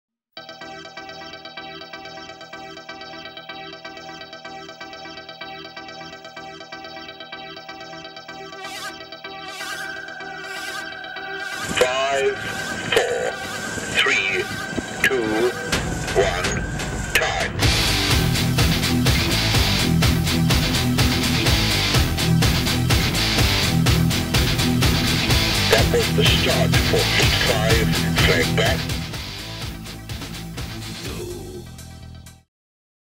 Nomad RIR 2011 Trailer
Nomad Sailing's team Growler battle it in the Round the Island Race 2011